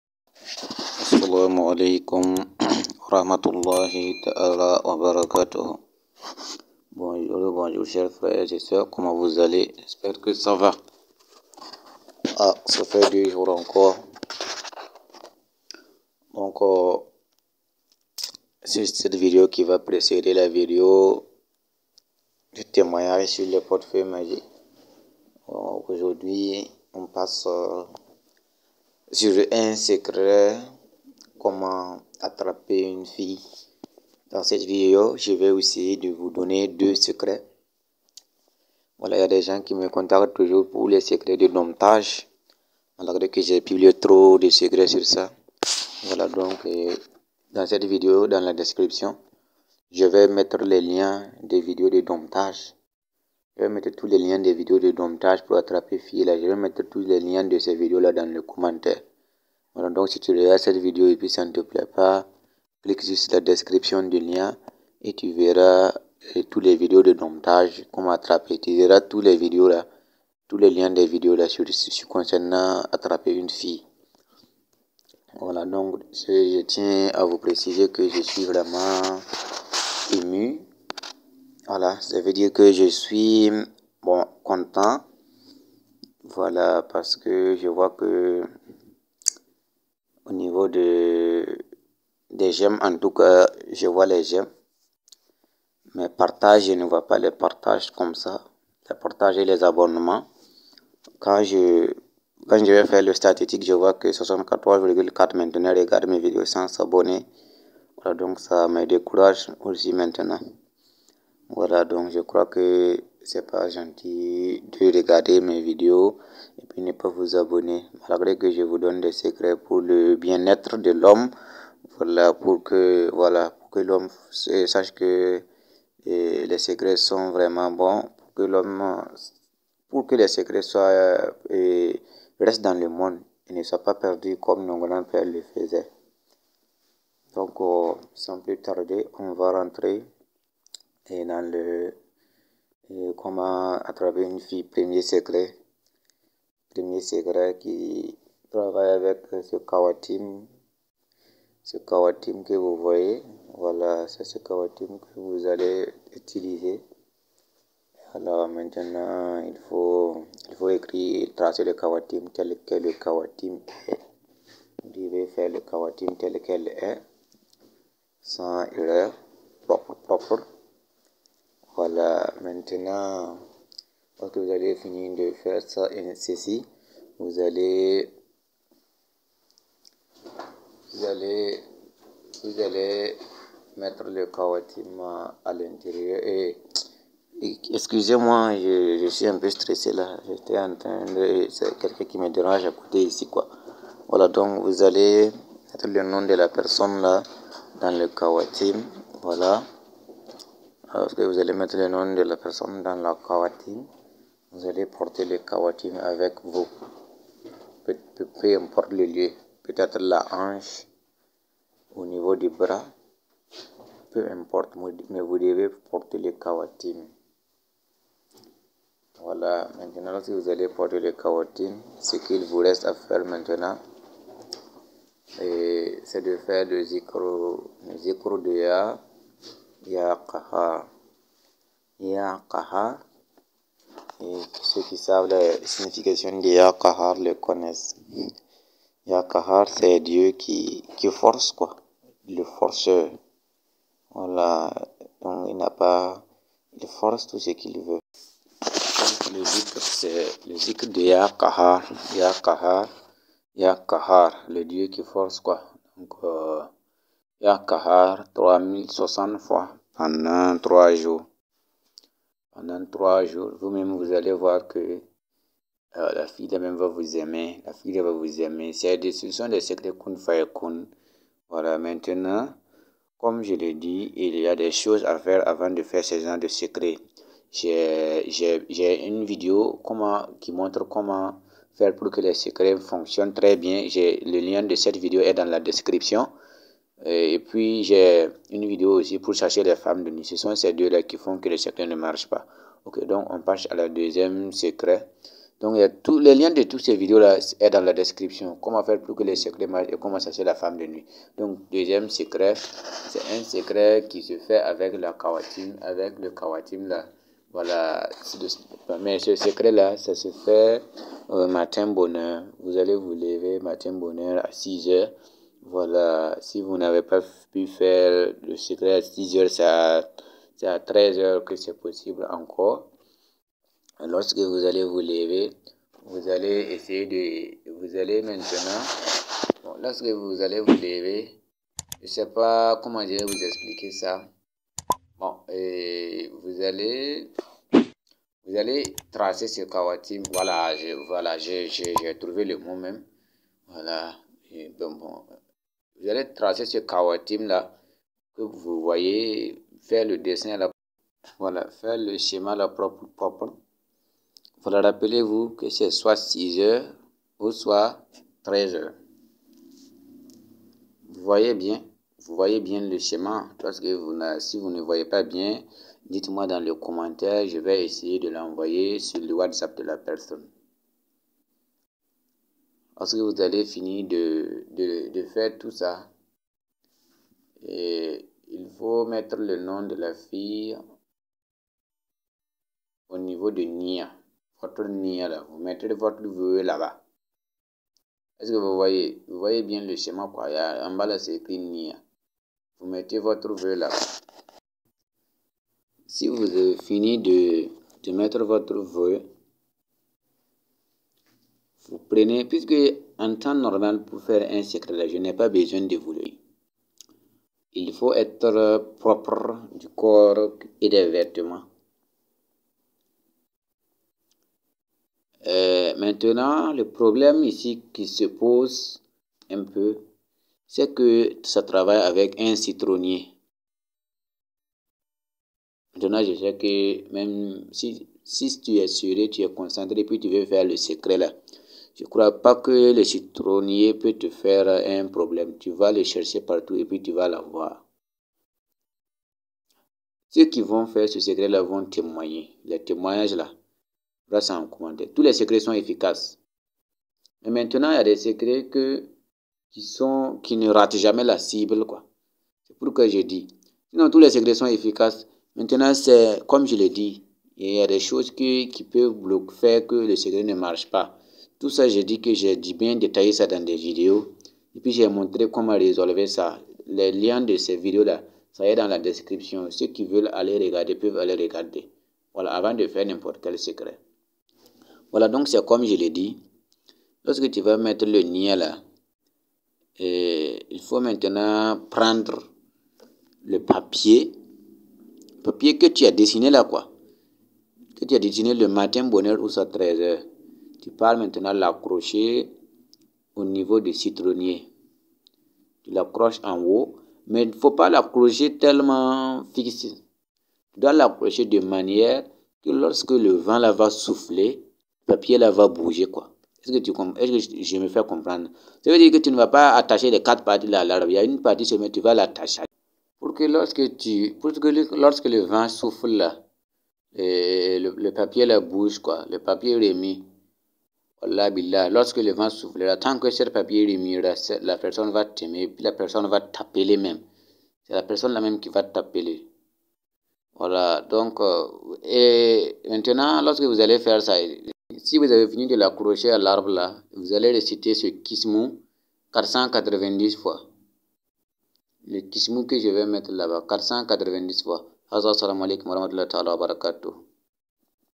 [DOMPTER] SECRET POUR DRAGUER OU ATTRAPER UNE FILLE OU FEMME
il s'agit de deux secret très efficaces pour attraper où faire revenir une fille ou une femme pour un bon amour et être aimer

👉 Pour chasser les femmes ou hommes de nuit

👉 Pour que les secrets fonctionne